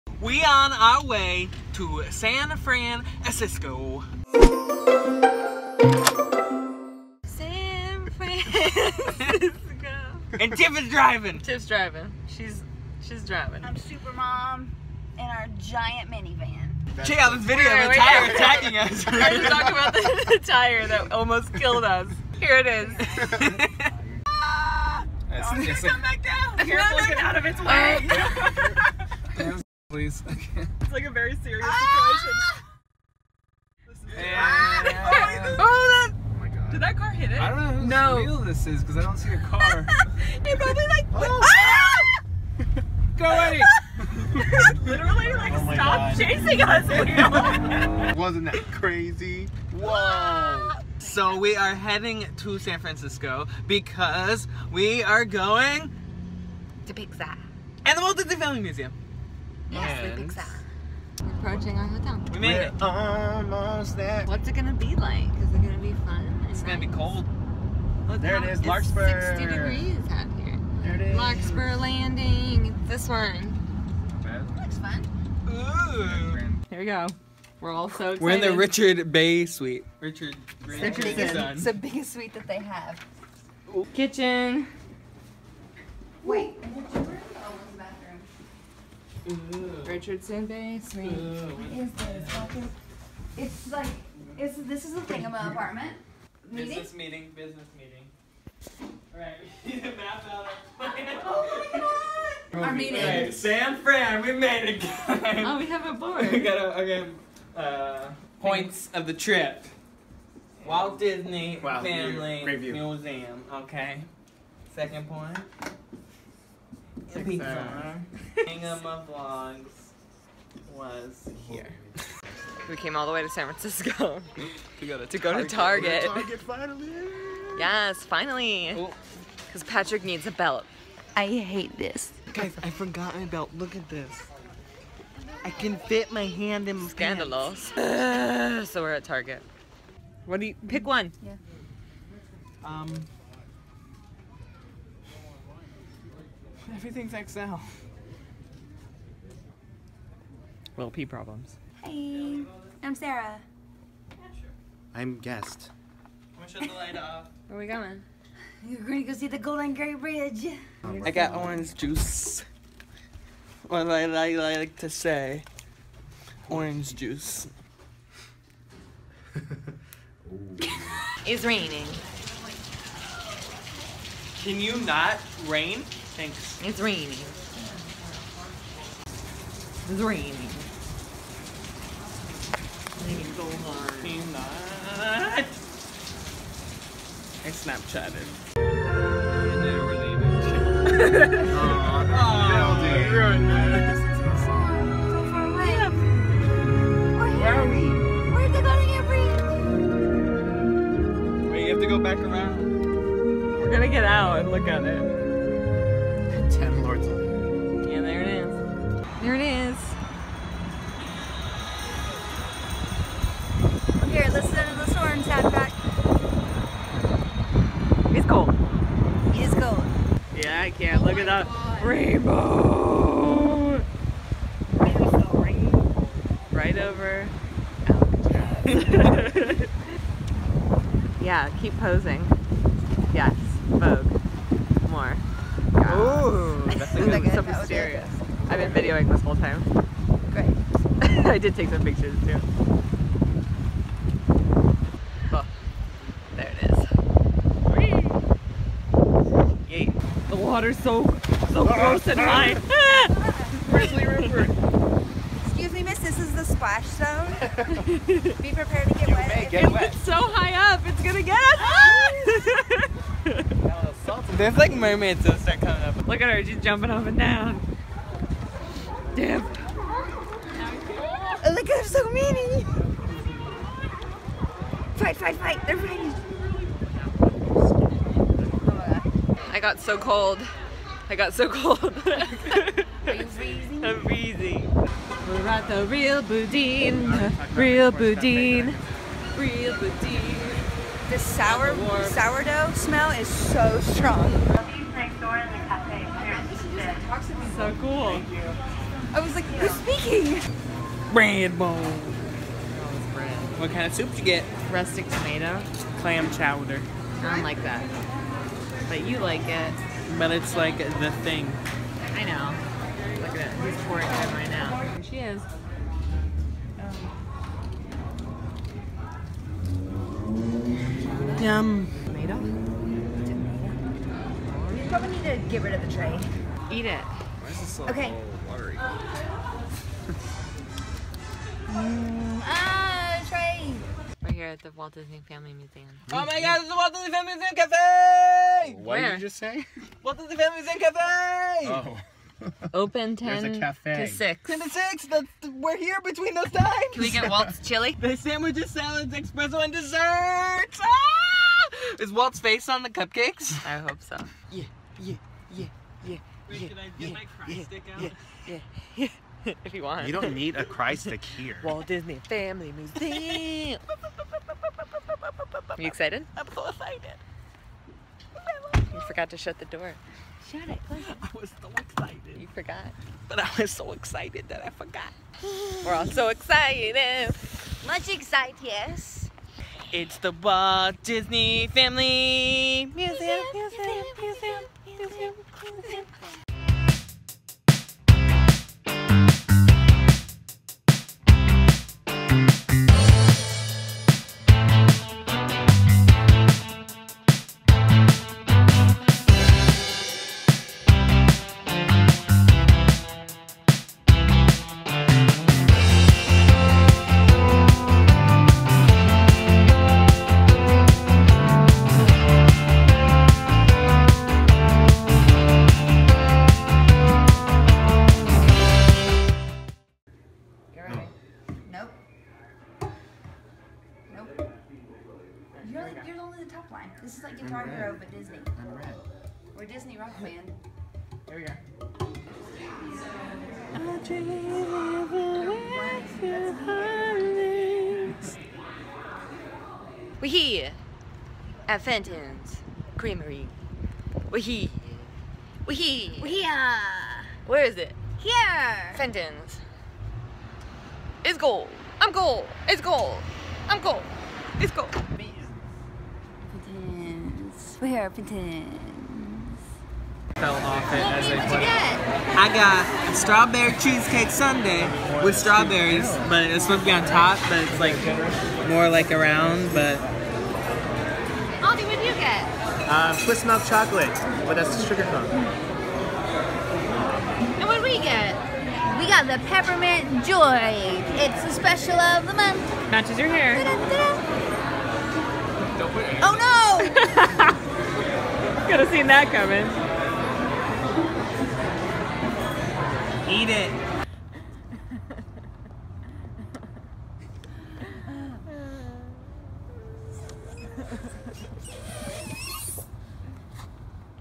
Thingamavlogs ROAD TRIP!
Join us on our road trip up to San Francisco where we did MANY exciting things, but primarily visited Pixar. Stay on the look out for a couple more videos coming in the next few days FROM Pixar!! Have you ever visited San Francisco?

Follow Thingamavlogs!